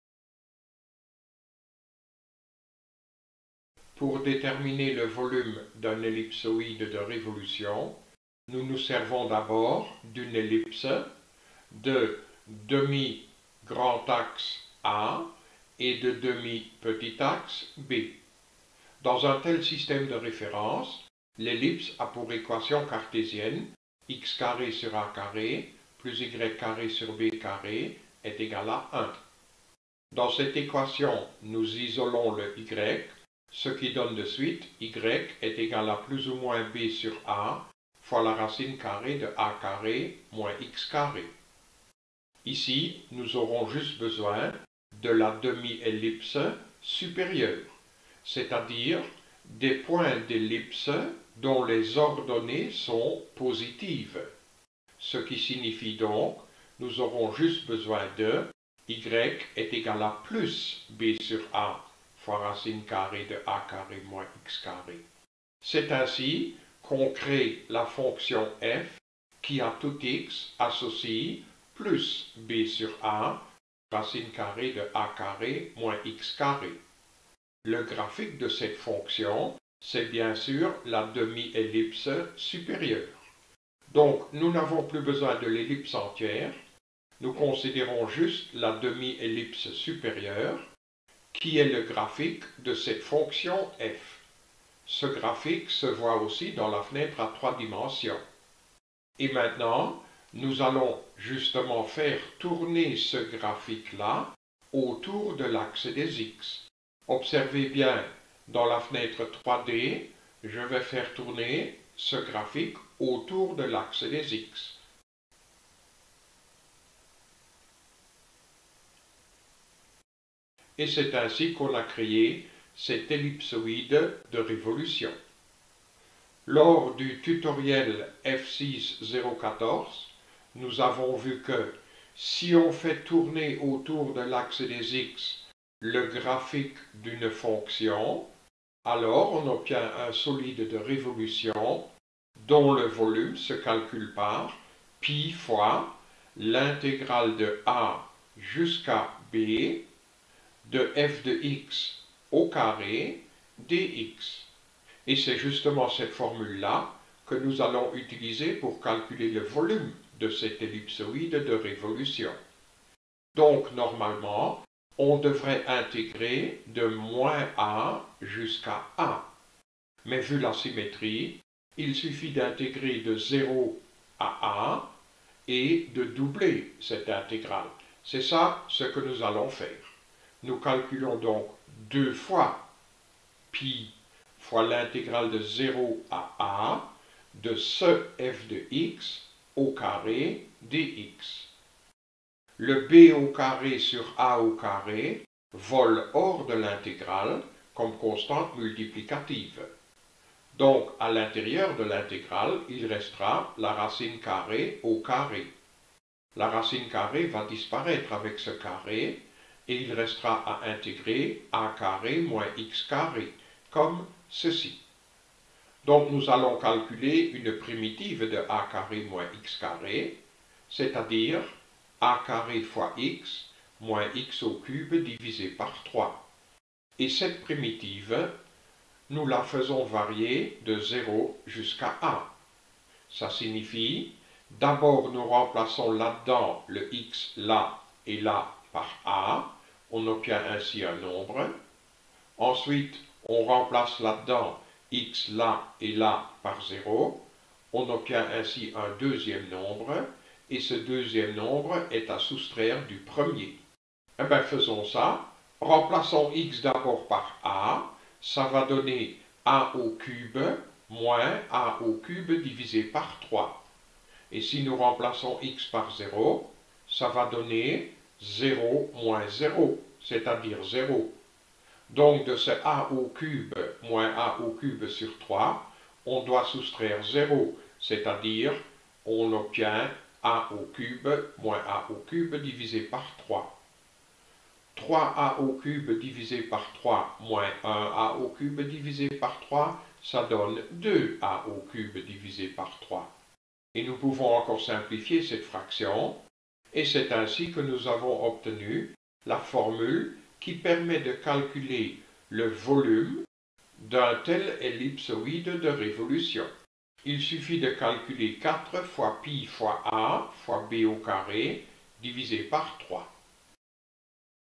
Intégrales 015
Nous calculons ici le volume d'un ellipsoïde de révolution par une intégrale.